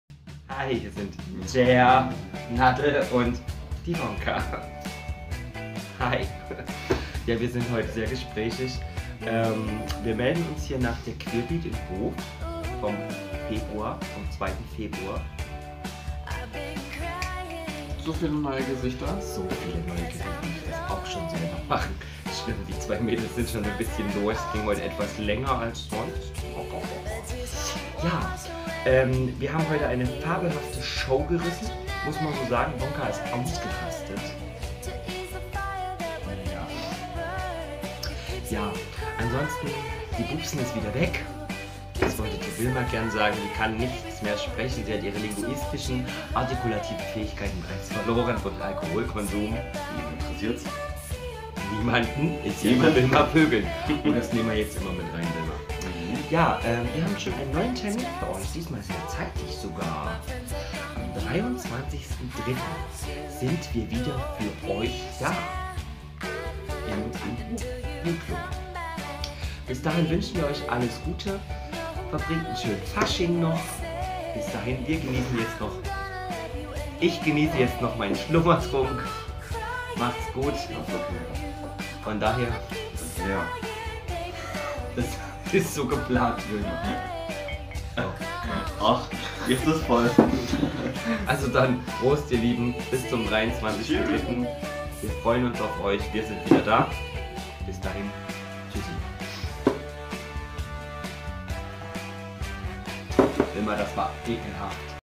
AfterShow am 2.2.19
Vonka ist ausgerastet und die wilma wiedermal ekelig! Wiedermal hat sich nichts verändert!